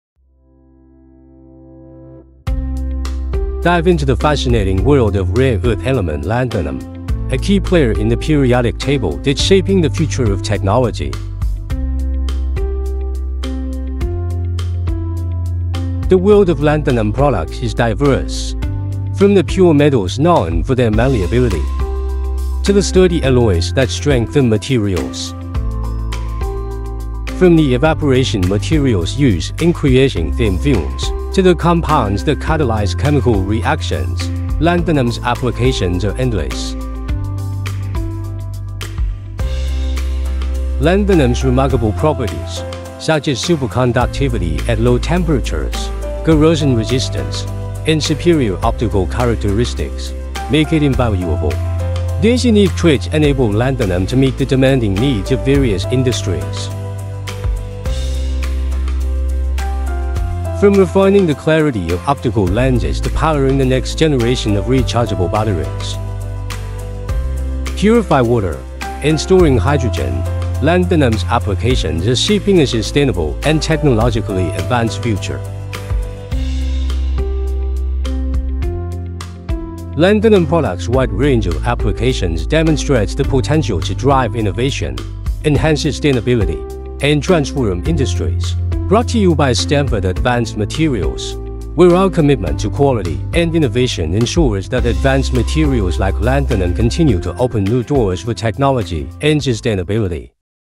The Versatile World of Lanthanum Products
Join us as we embark on a captivating journey into the world of Lanthanum, a rare earth element that stands at the forefront of technological innovation. From its key role in the periodic table to its diverse applications across industries, Lanthanum is not just a metal; it's a beacon of progress.

Dive deep into the essence of Lanthanum, exploring its pure metals known for their malleability, the strength of its alloys, the precision of its evaporation materials, and the catalytic power of its compounds. Discover how Lanthanum's unique properties like superconductivity, corrosion resistance, and superior optical characteristics make it indispensable in modern technology.

Witness its pivotal role in enhancing the clarity of optical lenses, powering revolutionary rechargeable batteries, purifying water, and storing hydrogen. This video is not just a tour through the applications of Lanthanum but a glimpse into a sustainable and technologically advanced future powered by Stanford Advanced Materials' commitment to quality and innovation.